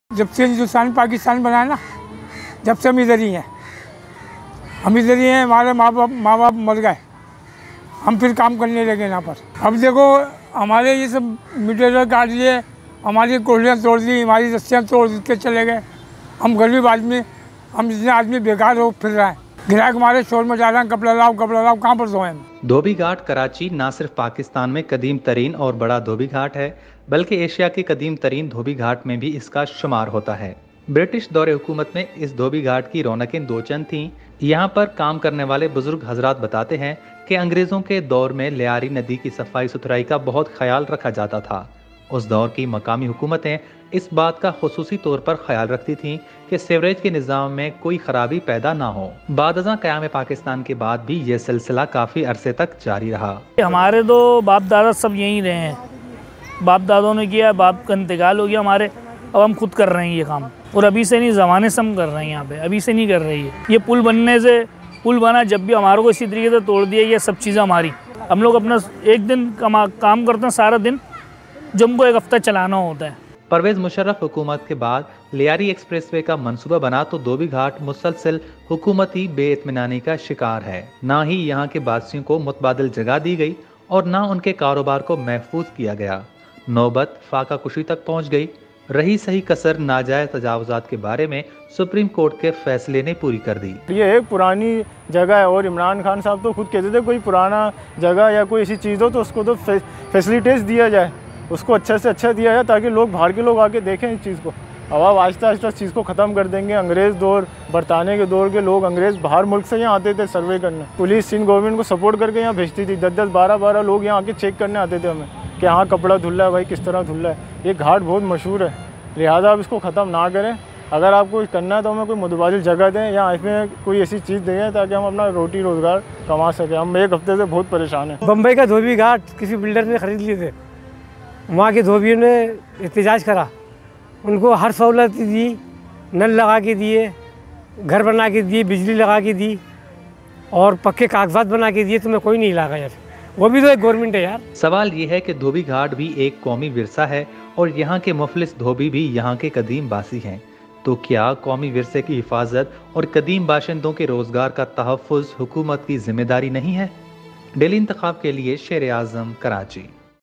Dhobi Ghat Karachi ab nhi raha | Washermen | Laundresses
Dhobi Ghat Karachi is considered to be one of the oldest Dhobi Ghats not only in Pakistan but also in Asia.The jobs of the poor laundresses and the ancient inhabitants are at stake.